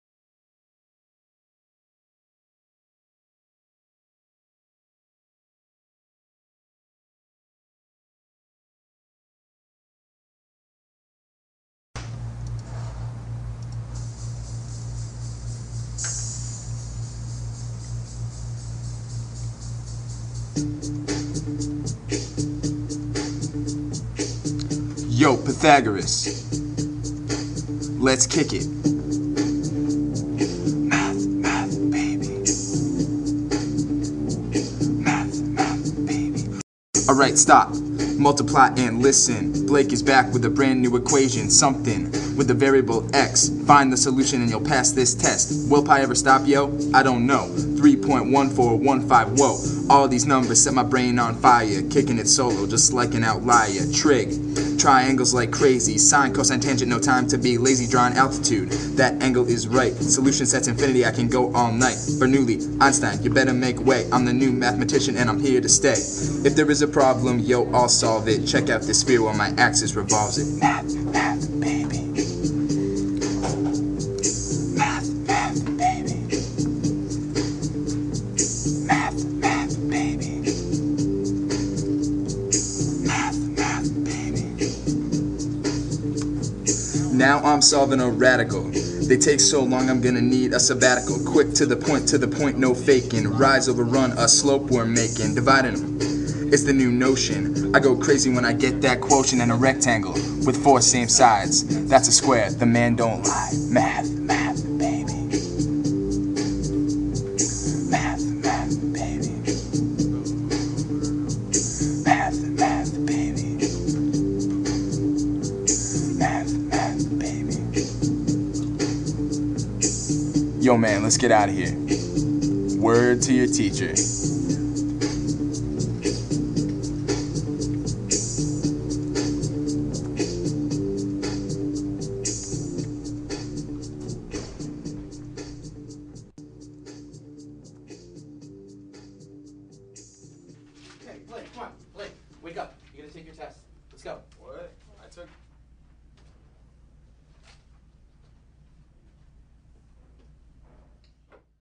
Math Math Baby
Blake M gives us a brief math lesson...